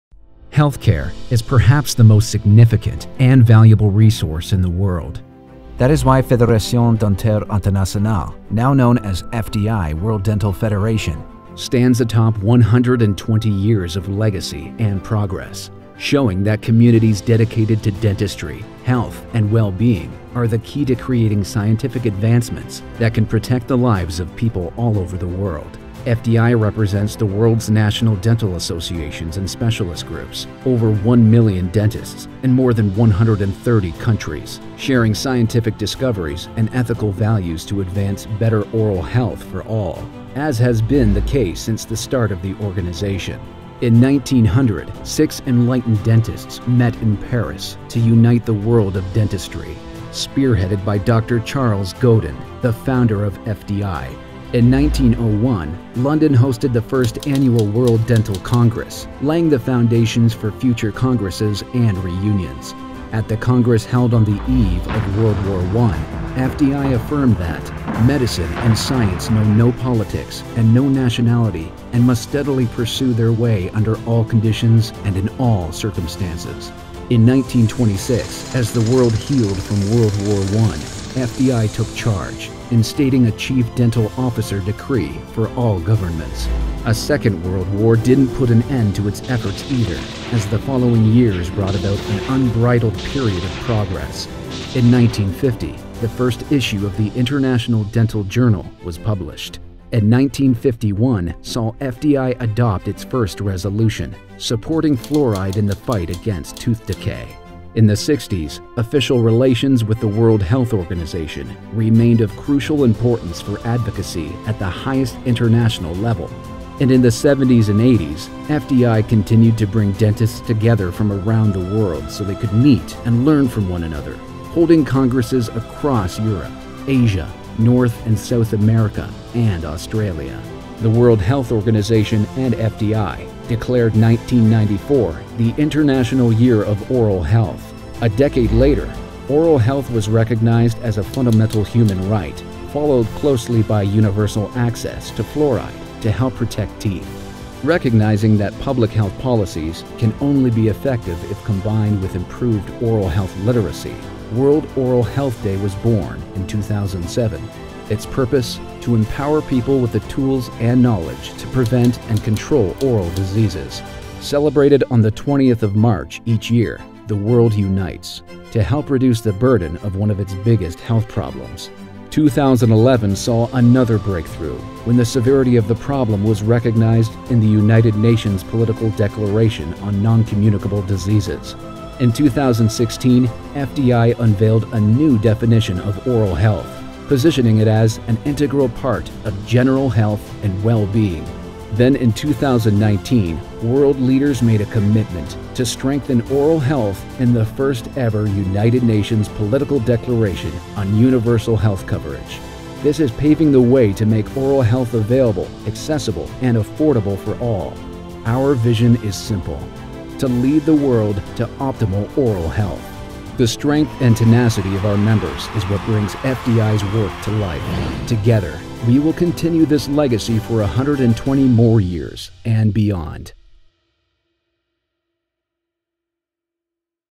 Happy 120th birthday, FDI World Dental Federation!
FDI's 120-year anniversary video details our ambitious debut in Paris in 1900 and details our resilience and many achievements for global oral health over the past twelve decades.